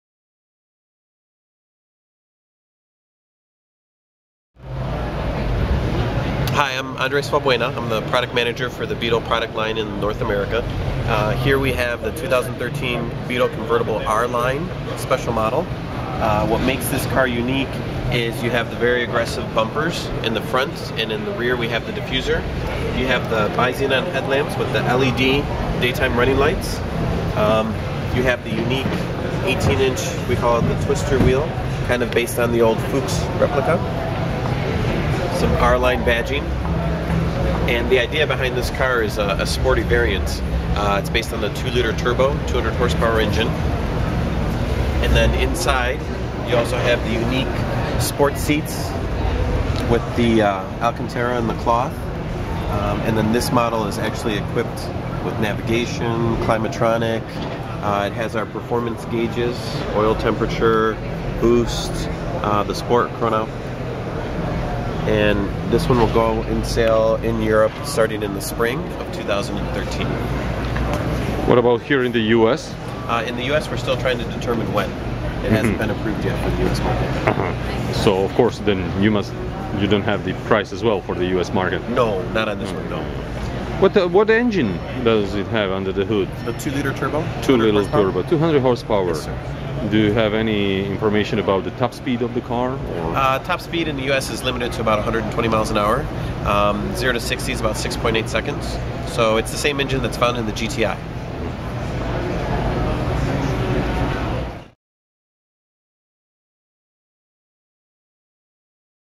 2013 Volkswagen Beetle Convertible R-Line - WORLD PREMIERE at LA Auto Show 2012 by KrekiLA
FirearmsGuide.com - gun encyclopedia on DVD - World Debut - 2013 Volkswagen Beetle Convertible R-Line - presented on 2012 Los Angeles Auto Show by KrekiLA